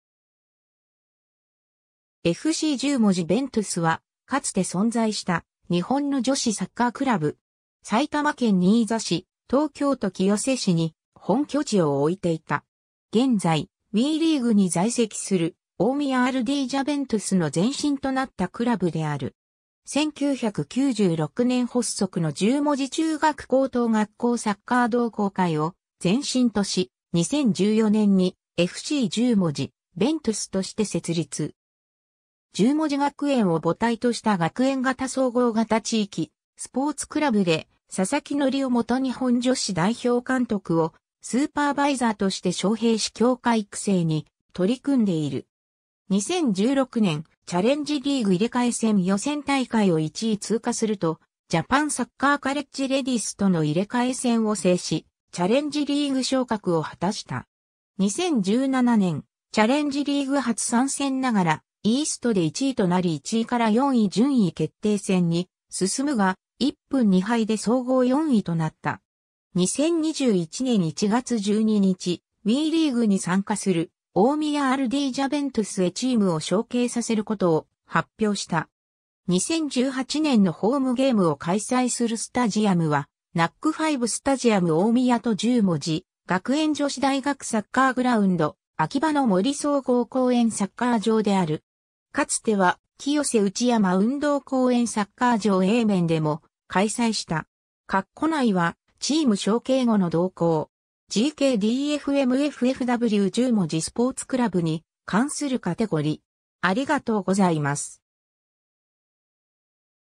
FC十文字VENTUS
FC十文字VENTUS（エフシーじゅうもんじベントス、FC Jumonji VENTUS）は、かつて存在した日本の女子サッカークラブ。
埼玉県新座市、東京都清瀬市に本拠地を置いていた。
 現在、WEリーグに在籍する大宮アルディージャVENTUS（大宮アルディージャの女子サッカー部門）の前身となったクラブである。
 1996年発足の十文字中学高等学校サッカー同好会を前身とし、2014年に「FC十文字VENTUS」として設立（VENTUSはラテン語で風）。
十文字学園を母体とした学園型総合型地域スポーツクラブで、佐々木則夫元日本女子代表監督をスーパーバイザーとして招聘し強化育成に取り組んでいる。
 2016年、チャレンジリーグ入替戦予選大会を1位通過すると、JAPANサッカーカレッジレディースとの入替戦を制しチャレンジリーグ昇格を果たした。
 2017年、チャレンジリーグ初参戦ながらEASTで1位となり1位-4位順位決定戦に進むが、1分2敗で総合4位となった。
 2021年1月12日、WEリーグに参加する大宮アルディージャVENTUSへチームを承継させることを発表した。
 2018年のホームゲームを開催するスタジアムは、NACK5スタジアム大宮（埼玉県さいたま市大宮区）と十文字学園女子大学サッカーグラウンド（埼玉県新座市）、秋葉の森総合公園サッカー場（埼玉県さいたま市西区）である。
 かつては、清瀬内山運動公園サッカー場A面（東京都清瀬市）でも開催した。
 カッコ内はチーム承継後の動向。
 GK DF MF FW 十文字スポーツクラブに関するカテゴリ: